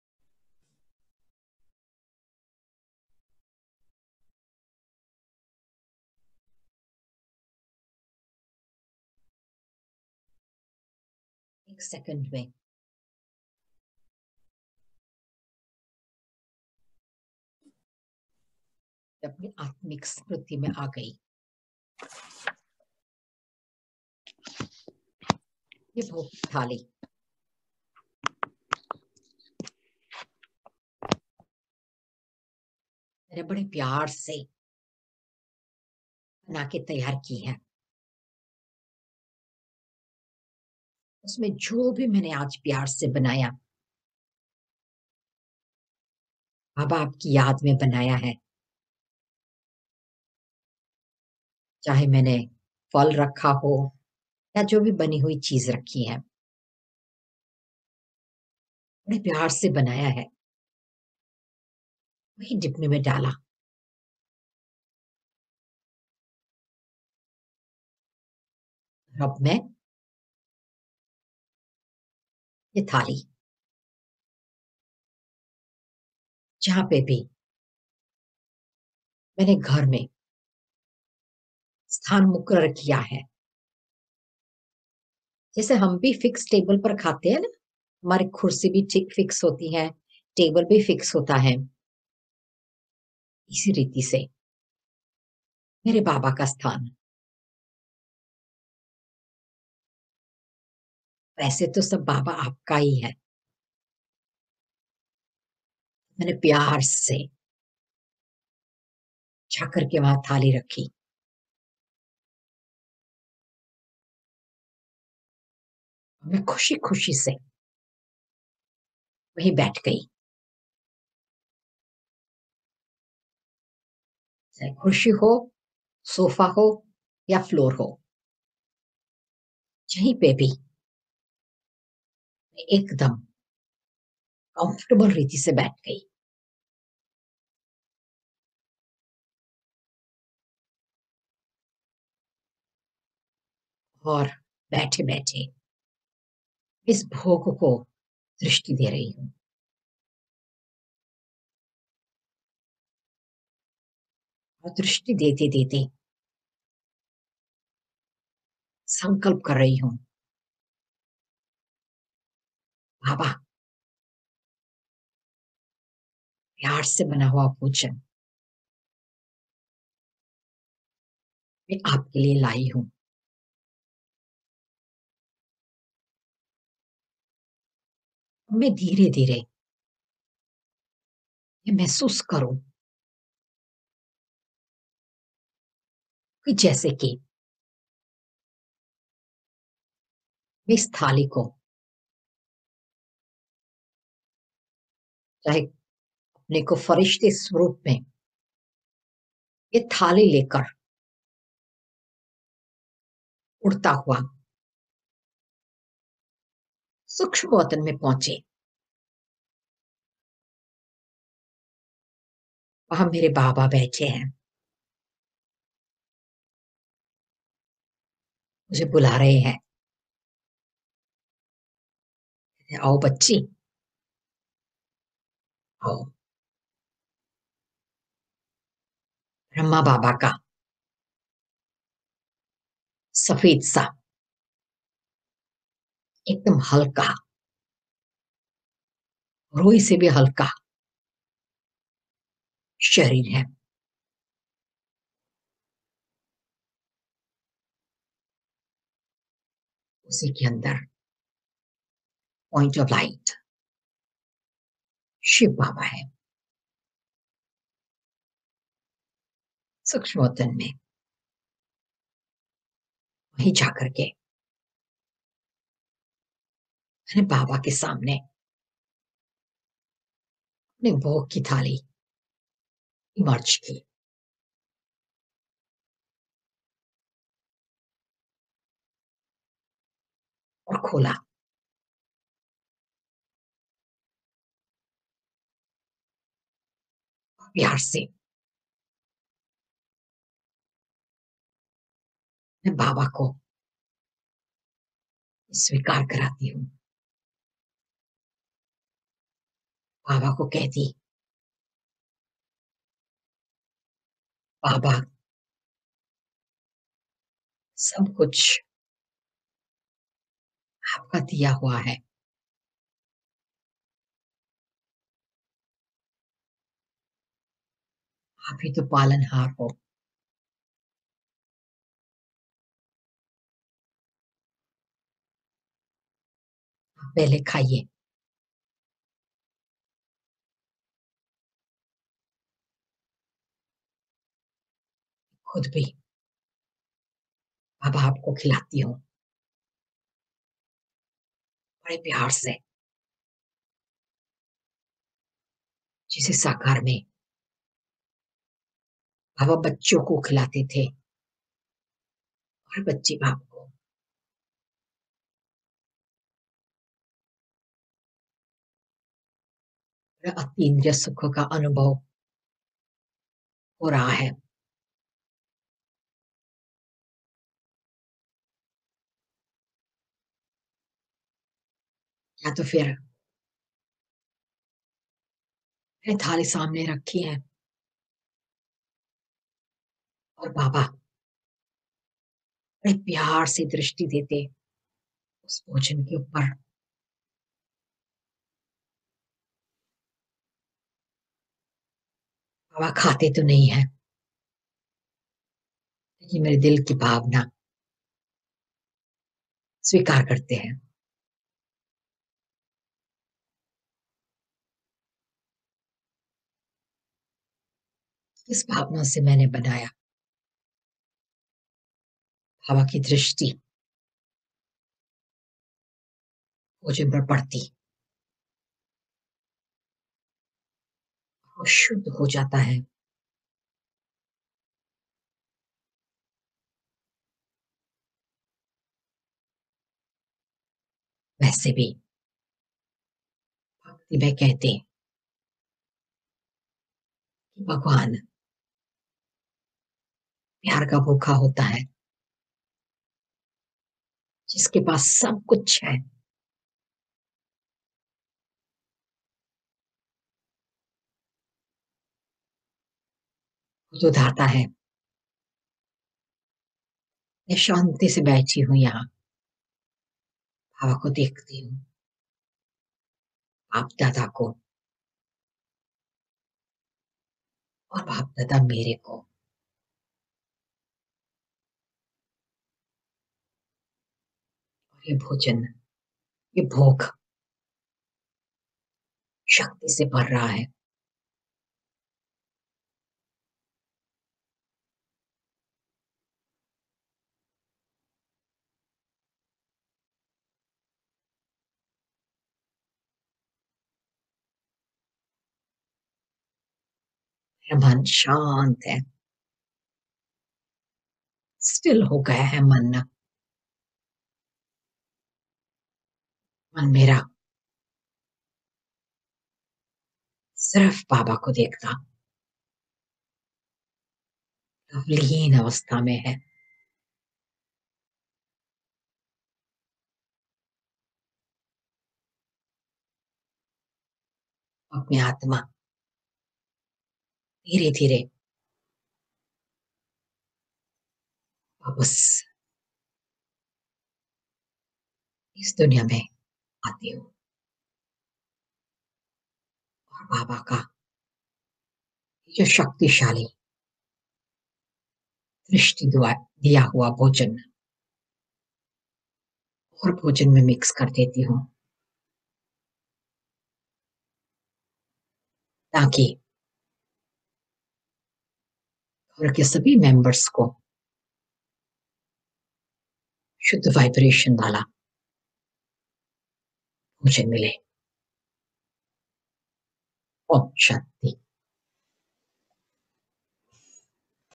Bhog Commentary with Sister Hema in Hindi
Anubhuti Retreat Center has a new Hindi Class Online with new students. So Sister Hema taught them a commentary experience for offering bhog.